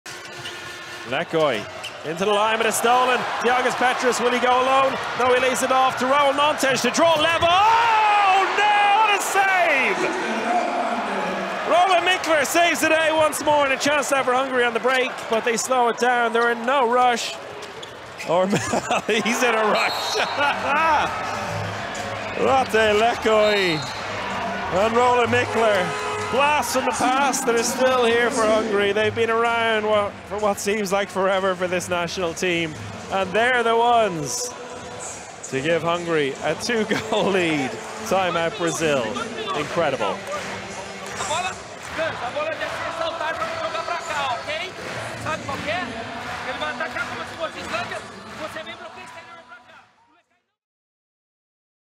The match-deciding moment 💥 Roland Mikler shuts down Brazil's comeback with this huge save
The match-deciding moment 💥 Roland Mikler shuts down Brazil's comeback with this huge save, paving the way for a critical win for 🇭🇺 Hungary 💪🏻